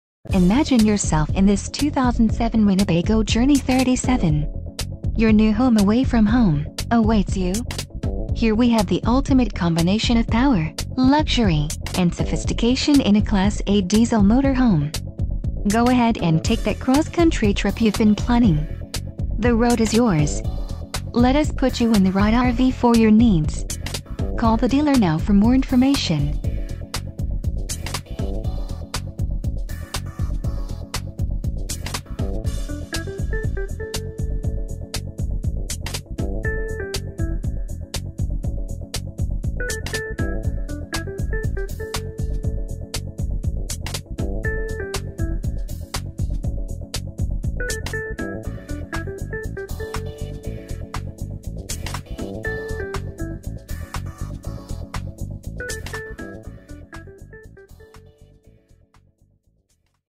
2007 Winnebago Journey - Orangewood RV Center - Suprise, AZ
Ever dream of waking up to a breath taking view, in this beautiful 2007 Winnebago Journey 37 model you can do just that! Boasting plenty of storage and space you will be able to unwind in this lightly used RV. Come into your local RV sales lot in Surprise today to get more information!